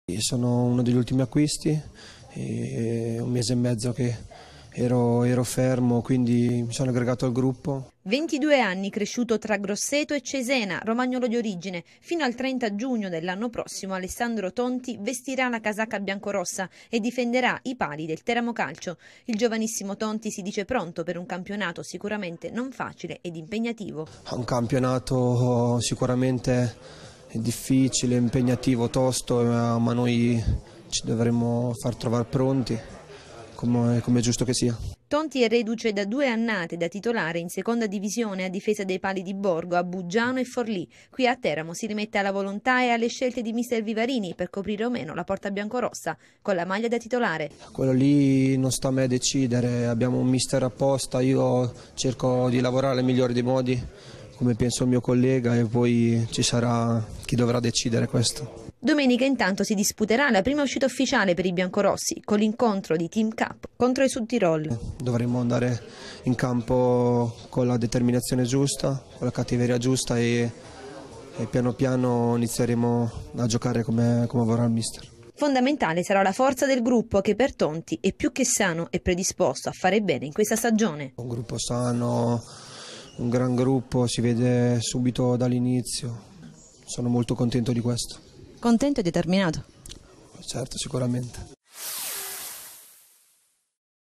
Intervista ad Alessandro Tonti, neo portiere biancorosso
Il canale ufficiale Youtube dedicato al Teramo Calcio